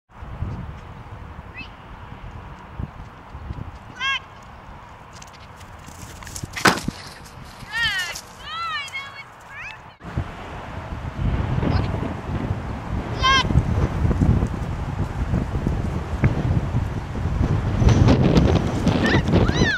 Jethro (Bloodhound) down while chasing a ball.
Jethro(Bloodhound) Lays down while chasing a ball, Rio (Mini Aussi) gets the ball after hitting the video camera.